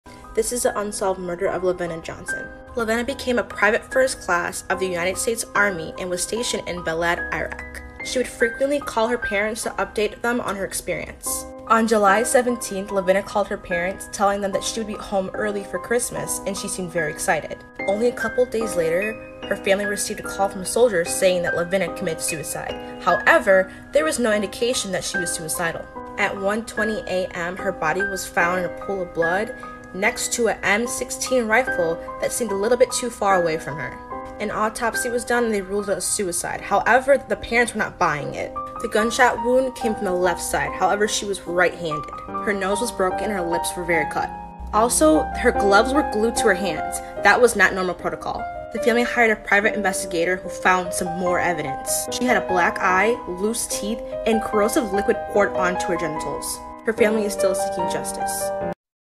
Quem Matou LaVena Johnson?
LaVena Johnson era uma soldada de primeira classe do exército norte americano que apareceu morta em sua tenda em um posto do exército no Iraque.

LaVena morreu em julho de 2005 e até hoje não se sabe ao certo o que aconteceu na noite de sua norte. A família de LaVena culpa o exército pois acreditam fortemente que eles estejam escondendo algo.

Esse Tiktok pertence a Kween_of_all_trad3s, você pode siga-la no tiktok, basta procurar pelo seu @.

A tradução e a legenda contida nesse vídeo foi inteiramente crida por nós aqui do canal.